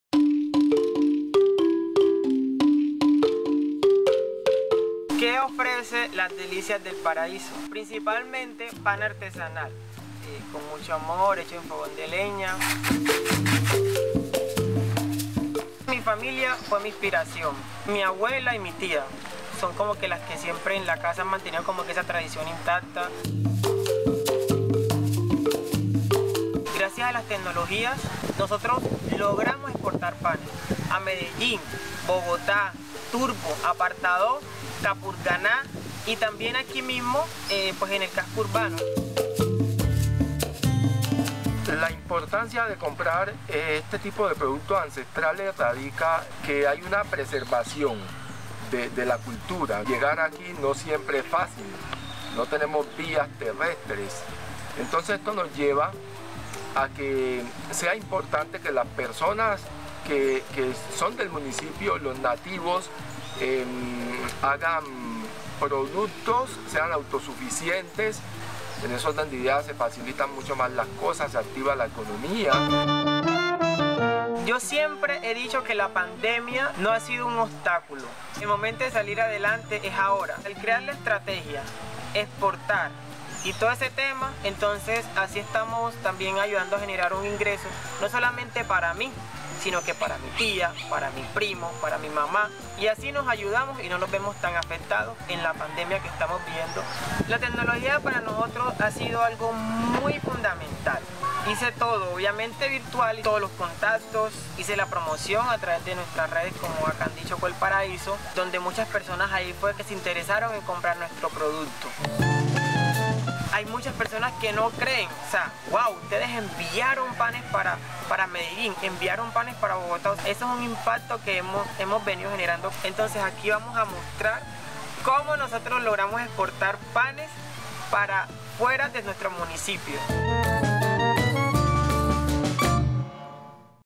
Acandi, la sucursal de las delicias del Paraíso
En Acandí, Chocó se encuentran “Las Delicias del Paraíso”, un emprendimiento familiar que busca mantener vivas las tradiciones gastronómicas de su pueblo y que gracias a la comercialización digital están deleitando a clientes de todas partes con sus productos.